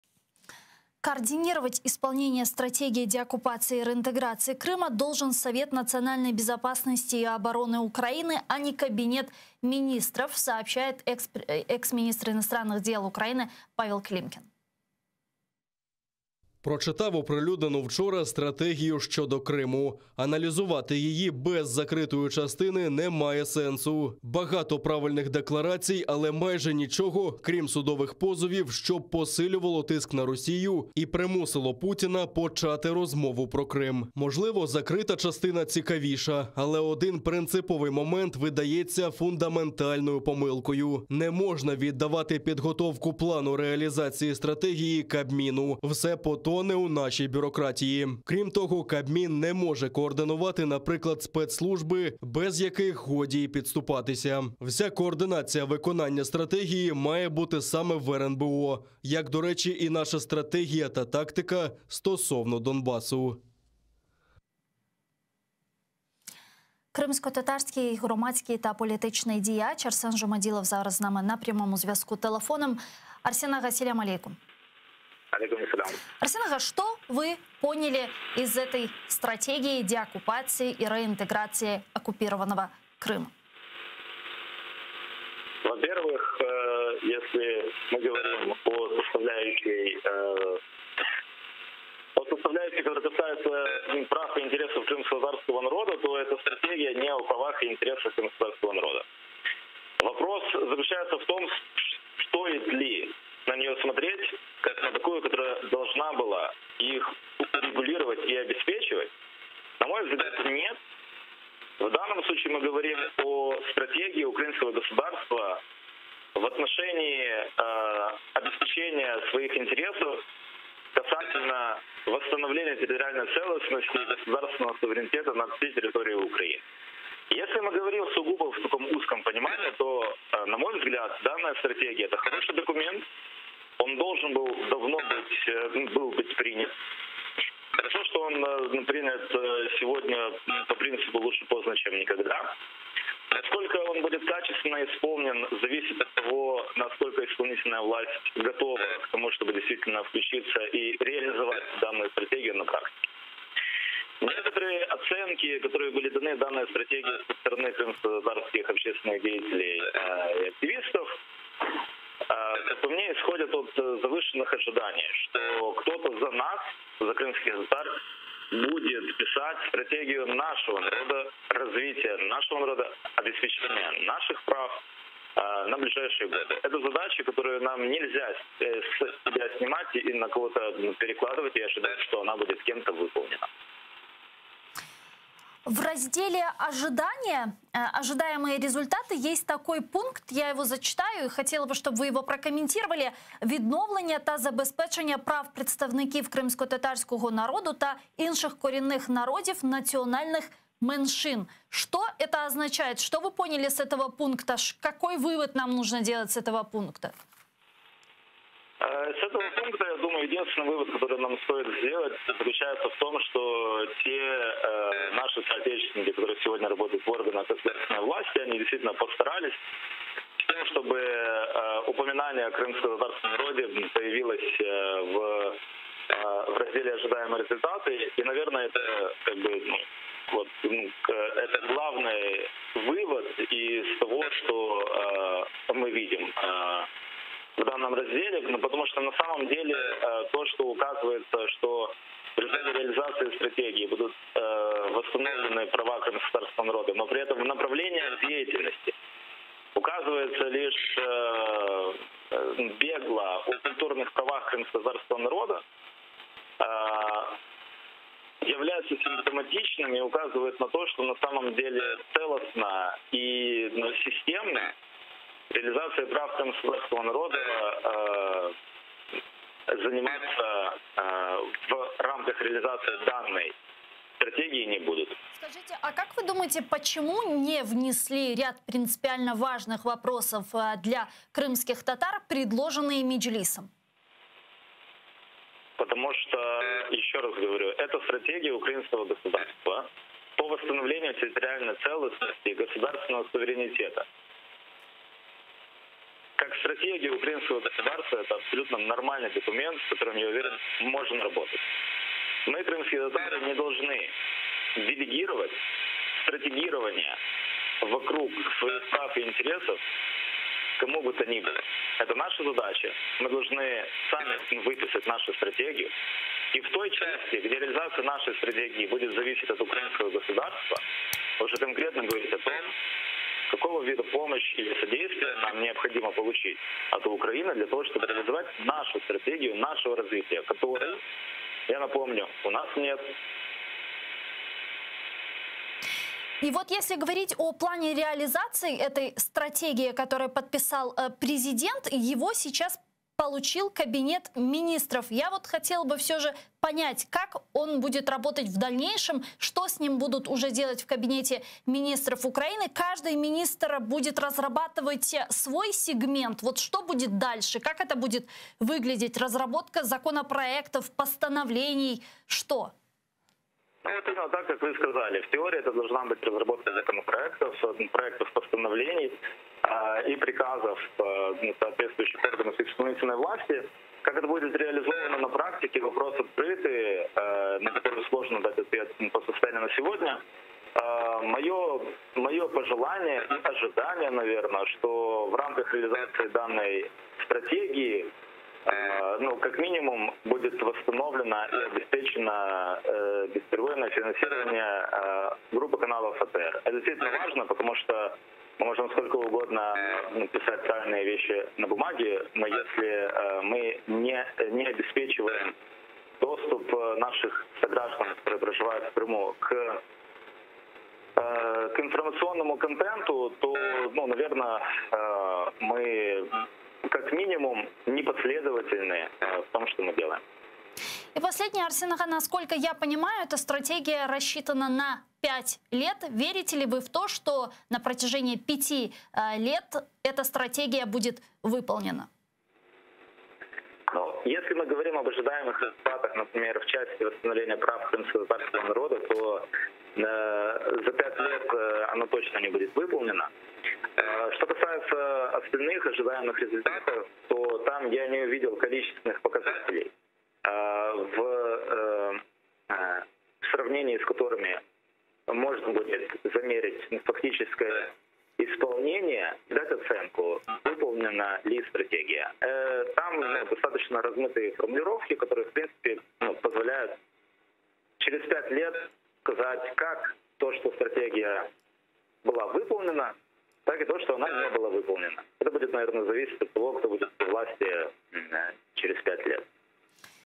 СТРАТЕГІЯ ДЕОКУПАЦІЇ КРИМУ “ПОТОНЕ” В БЮРОКРАТІЇ
Карта Монобанк:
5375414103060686

План реалізації стратегії деокупації Криму не можна доручати готувати Кабміну – Клімкін
Якщо Кабінету Міністрів України доручать підготовку плану реалізації стратегії деокупації Криму, усе "потоне" в бюрократії, вважає ексглава МЗС України Павло Клімкін.

«Прочитав опубліковану вчора стратегію щодо Криму. Аналізувати її без закритої частини немає сенсу, багато правильних декларацій, але майже нічого, крім судових позовів, що б посилювало тиск на Росію і змусило Путіна почати розмову про Крим. Можливо, закрита частина «цікавіше», але один принциповий момент представляється фундаментальною помилкою. Не можна віддавати підготовку плану реалізації стратегії Кабміну. Всі «потоне» в нашій бюрократії. Крім того, Кабмін не може координувати, наприклад спецслужби, без яких неможливо і підступатися. Вся координація виконання стратегії має бути саме в РНБО, як, до речі, і наша стратегія і тактика щодо Донбасу», – написав Клімкін в Фейсбуці.

Коментує Арсен Жумаділов — кримськотатарський громадський та політичний діяч